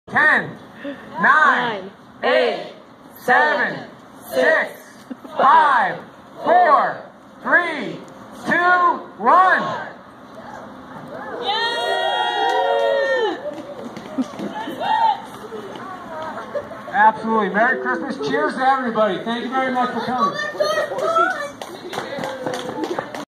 Brewery Lights at Budweiser in Fort Collins, Colorado
See the BreweryLights go on at the Brewery Experience in Fort Collins at Anheuser Busch. Super fun event that runs through December 30, 2016.